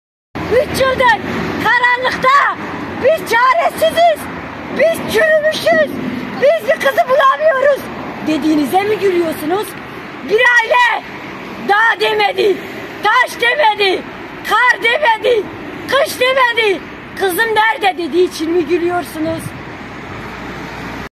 '300 yıl geçse de Gülistan'ı aramaktan vazgeçmeyeceğiz'
5 Ocak 2020 tarihinden bu yana kendisinden haber alınamayan Gülistan Doku'nun ailesi Tunceli Adliyesi önünde adalet arayışını yineledi